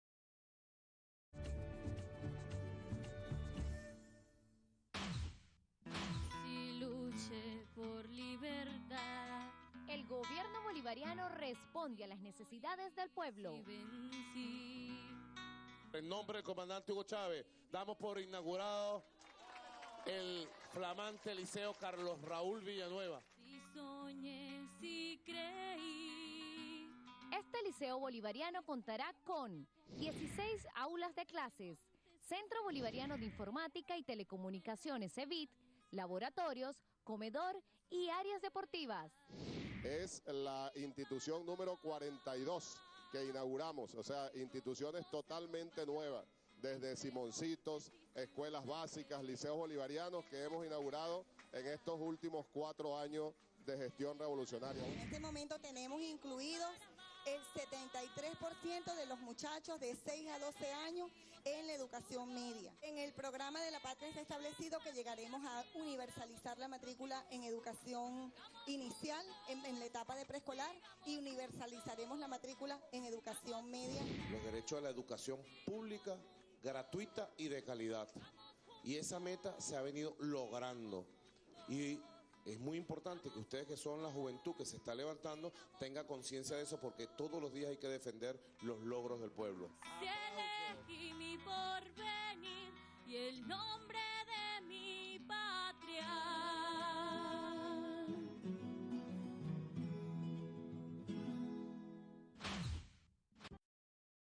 Micro Inauguración Liceo Bolivariano Carlos Raúl Villanueva
Micro de VTV Inauguración Liceo Bolivariano Carlos Raúl Villanueva ubicado en el estado Barinas